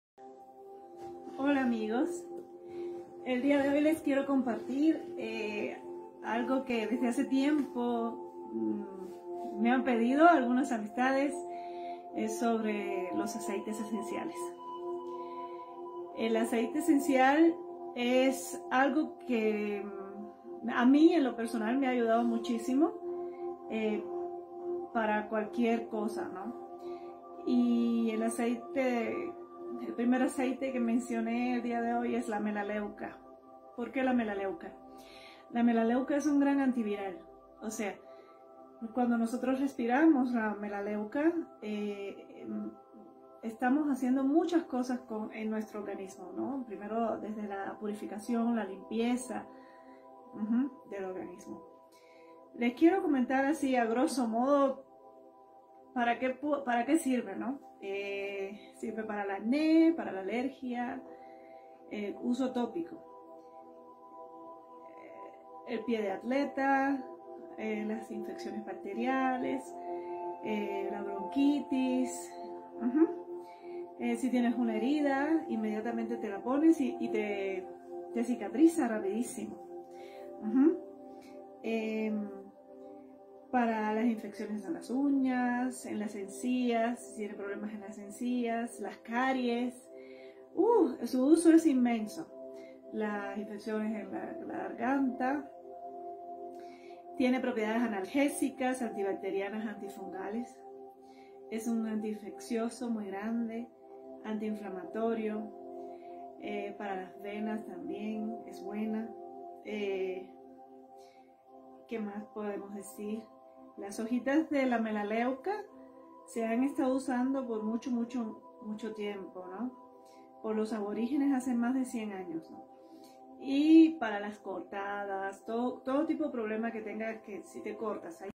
Aceite esencial Melaleuca
Reconocido por su efecto limpiador y
rejuvenecedor en la piel
• Promueve la función inmunológica
saludable*
• Protege contra las amenazas ambientales
y estacionales*